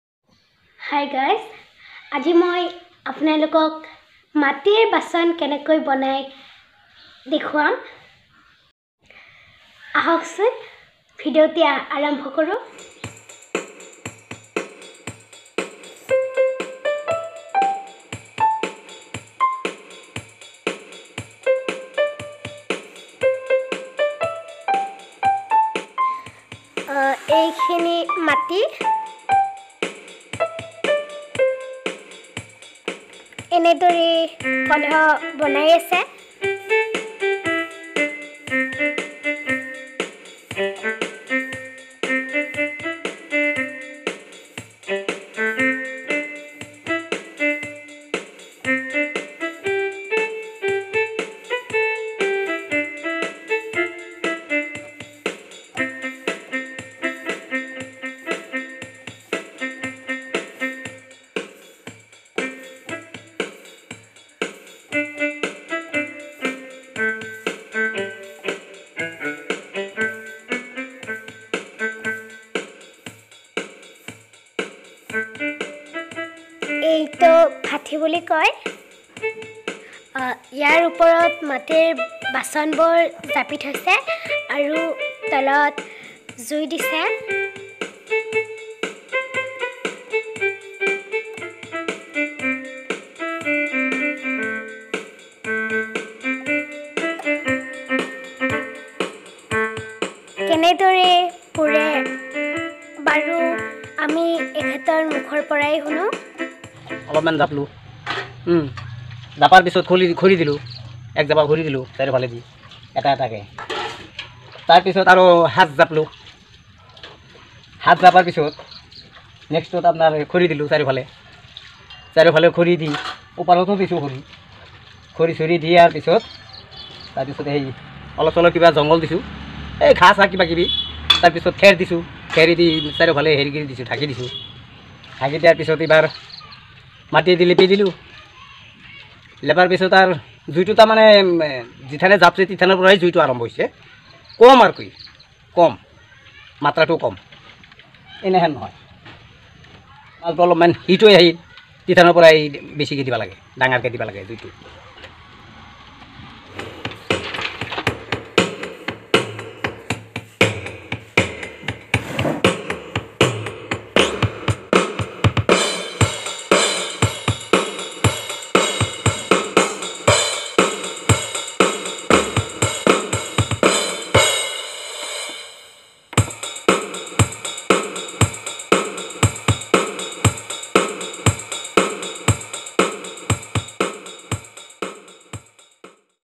মাটিৰ বাচন=বৰ্তন কেনেকৈ বনায় চাও আহক ৷৷
কুমাৰসকলে কেনেকৈ বনায় মাটিৰ বাচন=বৰ্তন ৷৷